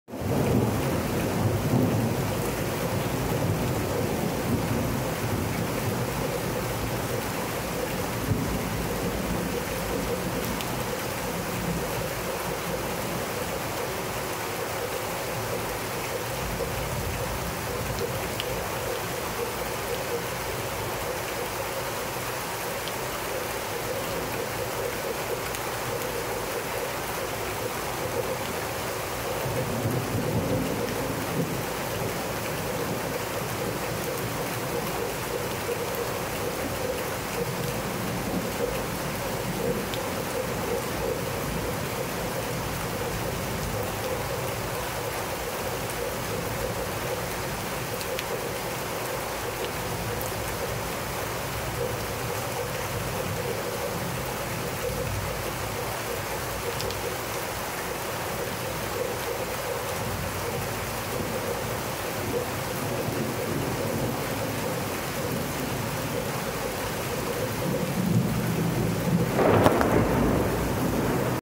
Calming relaxing rain and thunder The Woodlands Texas
Calming and relaxing sounds and Rain Live From The Woodlands Texas